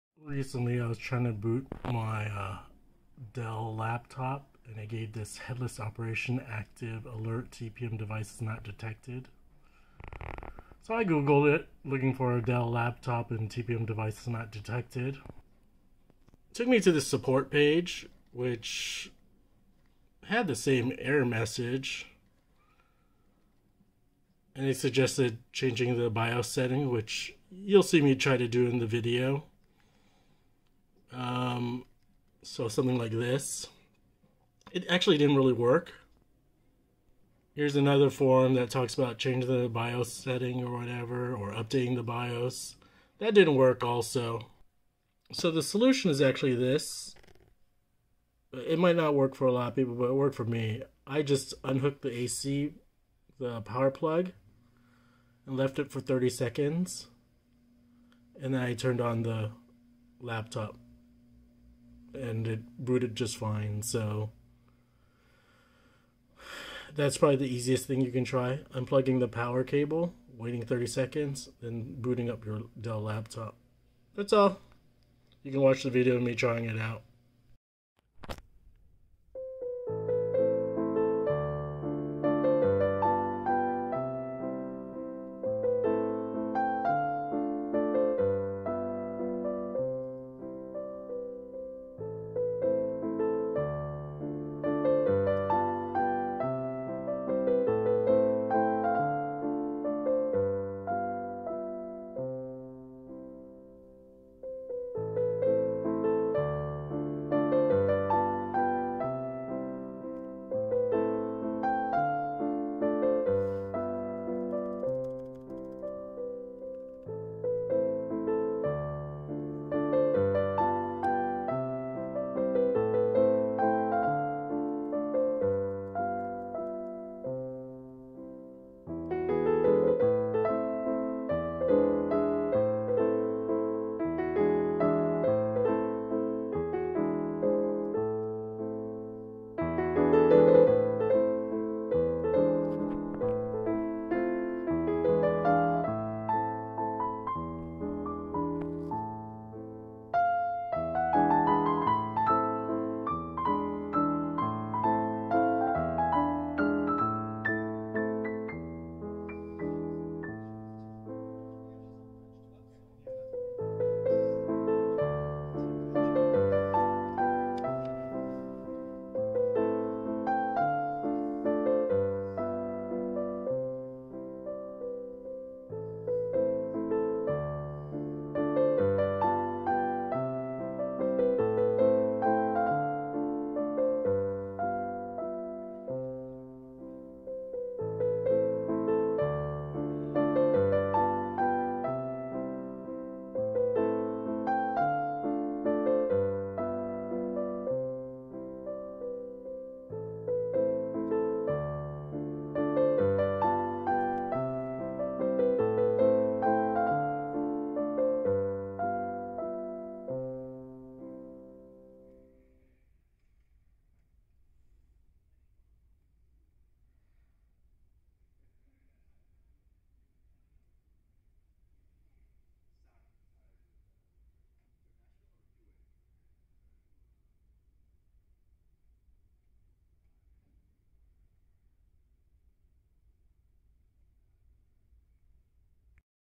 Alert! TPM device is not detected. Dell laptop. Removed AC Power for 30 sec. Turned on Laptop
I received this error recently on my dell laptop.

HEADLESS OPERATION ACTIVE
Alert! TPM device is not detected.

It would not allow me to boot into the windows 10 OS.

2.  I read this...

3.  Finally, in part of this forum, it mentions removing the power and waiting 30 seconds. This worked for me:

Good luck.  I'm not sure what solution will work for you.. but I hope you don't run into this error.